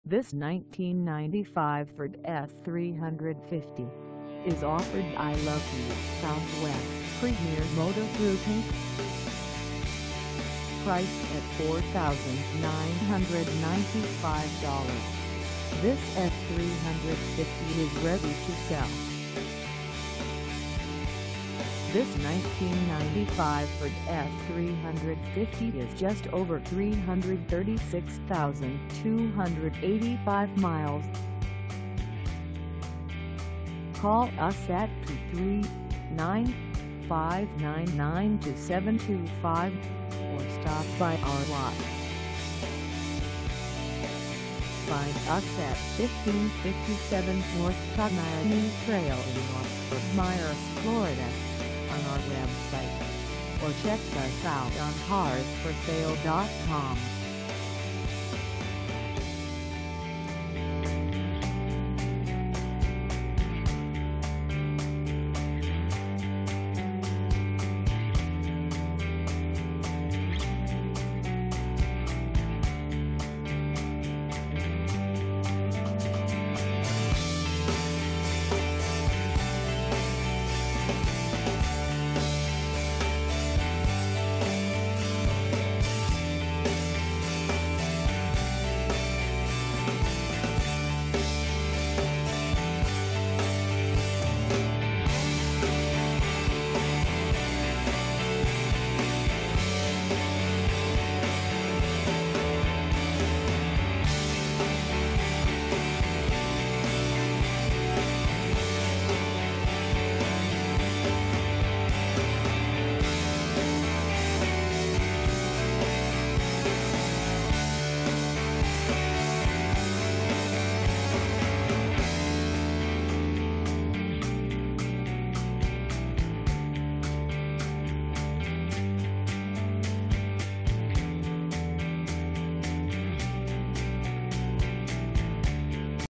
1995 Ford F-350 Long Bed for sale in North Fort Myers, FL 33
This 1995 Ford F-350 Long Bed is for sale in North Fort Myers, FL 33903 at Lucky's SW Premier Motor Group Inc.

Engine: 7.3L V8
Transmission: Automatic
Color: BLUE
Mileage: 336285
Contact Lucky's SW Premier Motor Group Inc 1557 North Tamiami Trail North Fort Myers, FL 33903 to test drive this 1995 Ford F-350 Long Bed today.